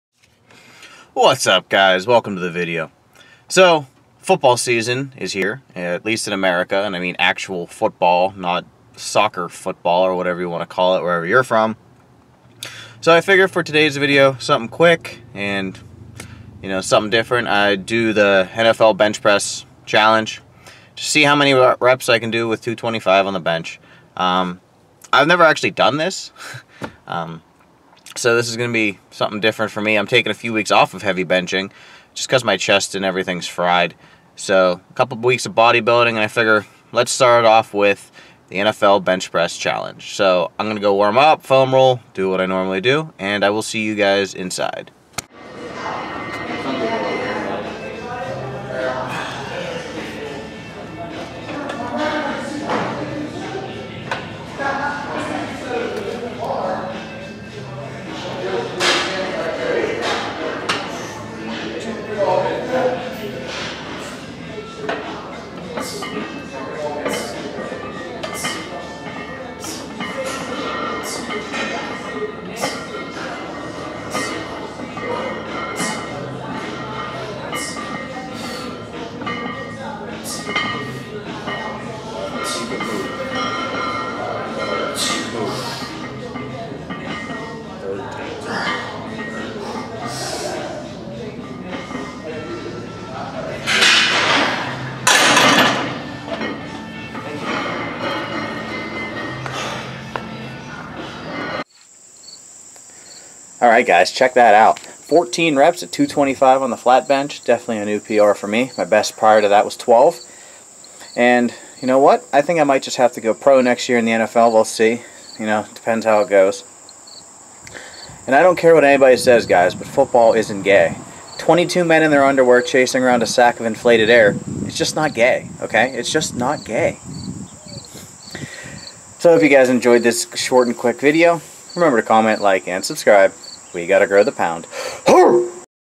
NFL Combine Bench Challenge
I haven't attempted an all out AMRAP set with 225 for quite some time. And, with the NFL preseason starting up, I figured it was time to see where I stand. How did I do?

Camera used: GoPro 5